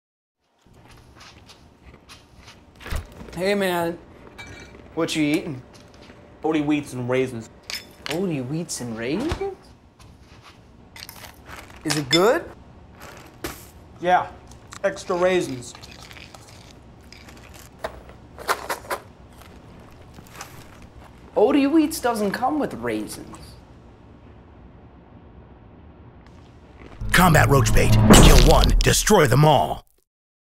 Combat Roach Bait 'Good Morning' SPEC Commercial
This hilarious spot is a SPEC commercial directed by David Weinstock (weinstockny.com) and produced by Waif & Stray. It features Christopher Van Jura (christophervanjura.com) and Barry McBrien. This commercial has not been purchased yet - but if the good people at Combat were smart they'd scoop it up now!